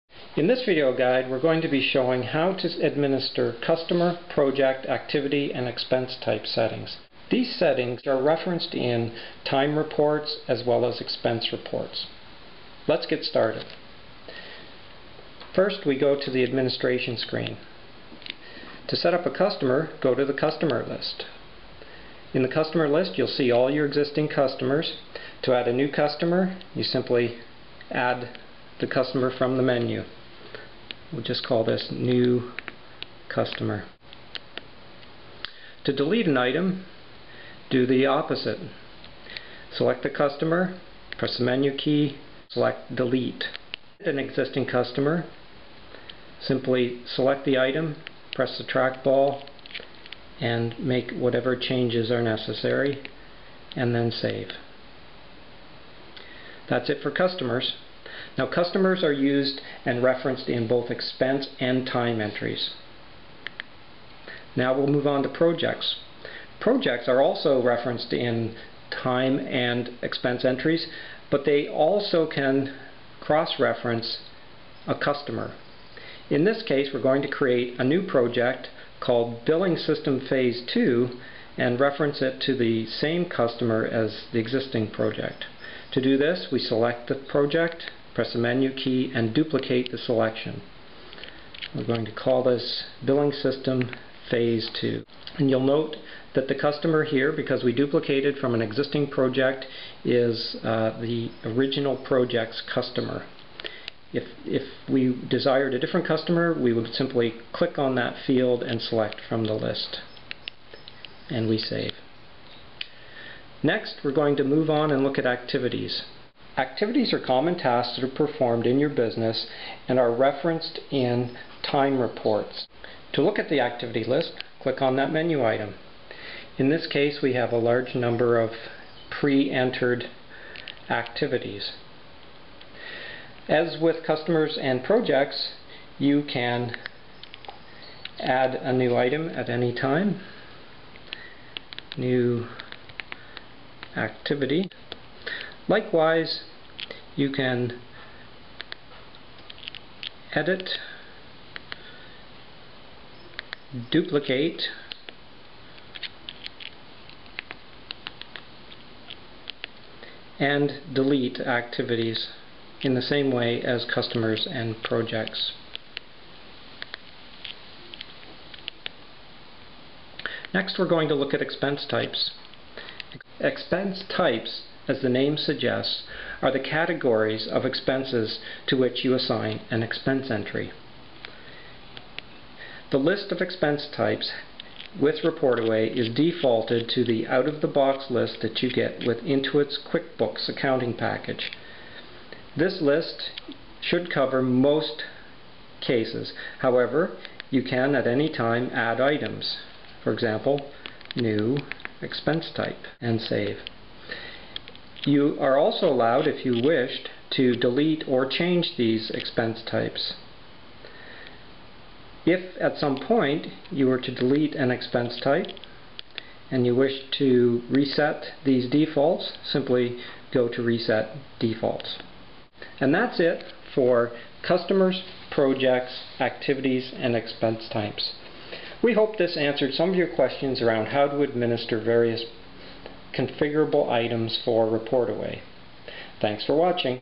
ReportAway Administration R1_3
This video guide shows how to configure Customers, Projects, Activities, and Expense Types for ReportAway! .